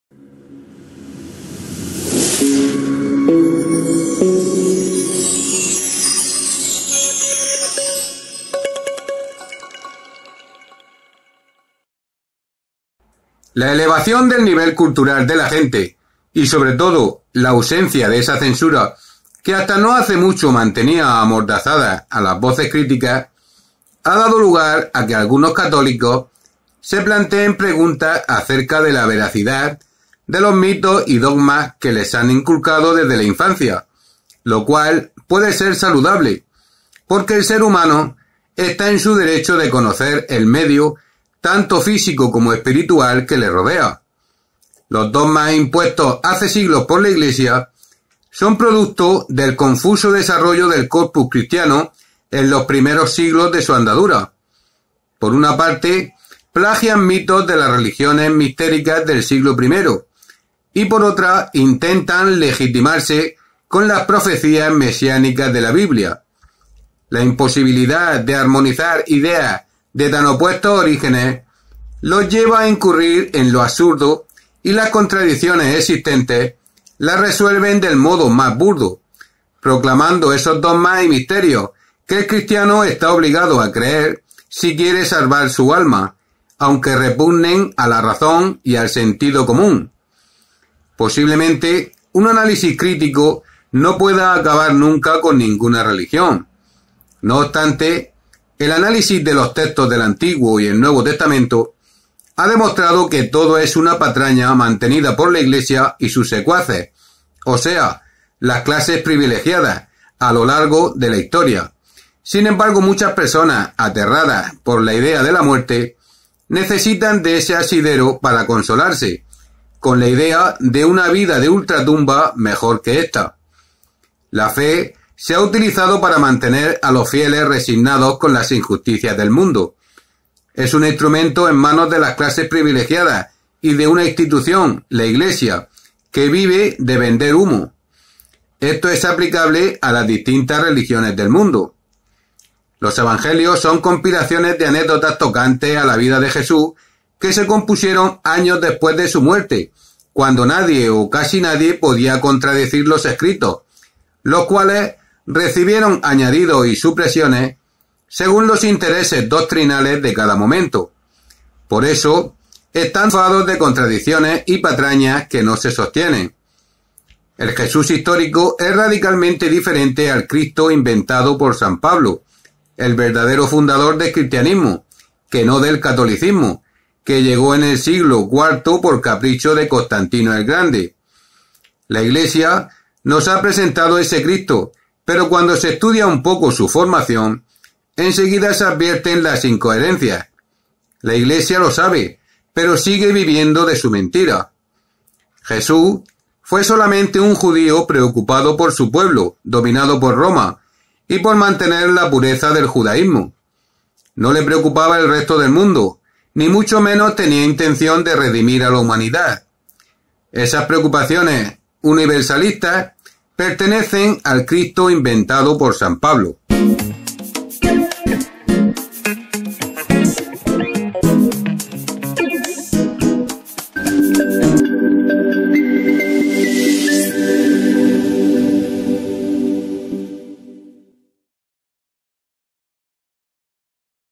El Cristo inventado por San Pablo
El Jesús histórico, es radicalmente diferente al Cristo inventado por San Pablo, el verdadero fundador del cristianismo, que no del catolicismo, que llegó en el siglo IV por capricho de Constantino el Grande.

17/01/2023